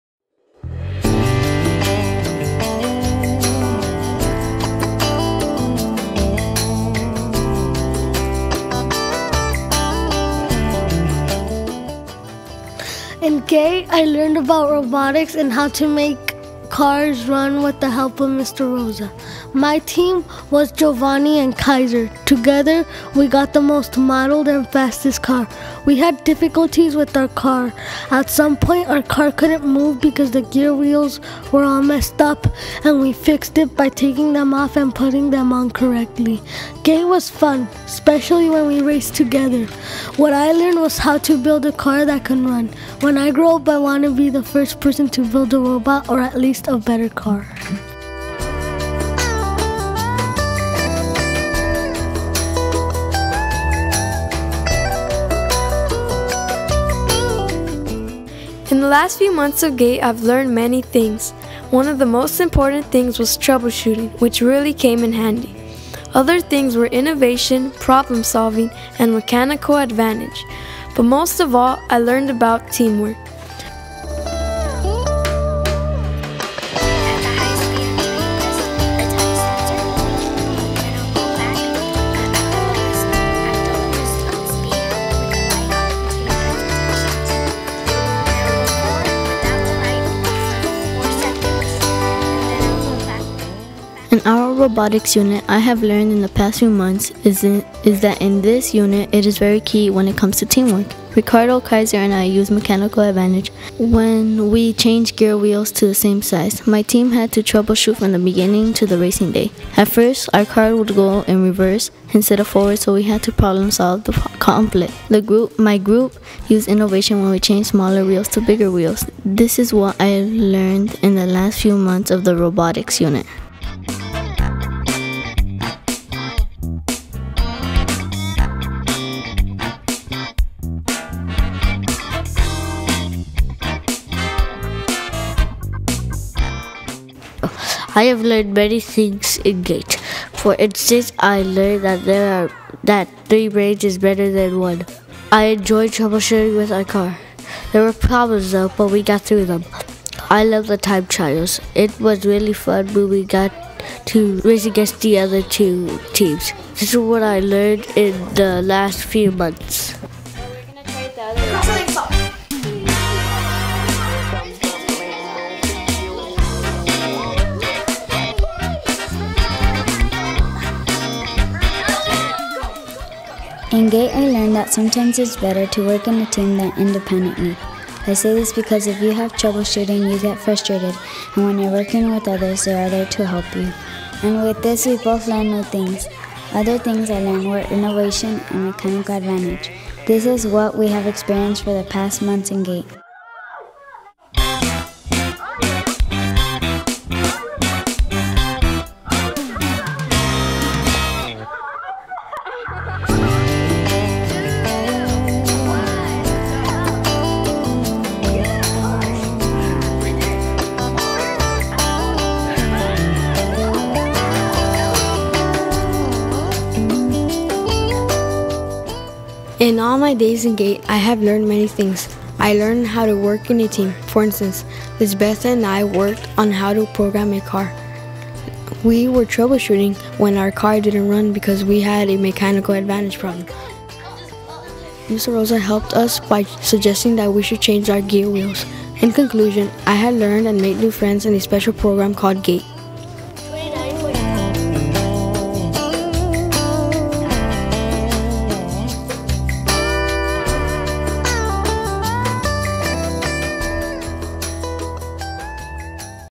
Evergreen GATE Lego Robotics
Evergreen's GATE students from 4th and 5th grade practiced programming and problem-solving during our Lego Robotics class this spring. The time trials finale was the best part with each team trying to improve it's best time.

Video by Cliff Rosa "Mr. Rosa 2.0"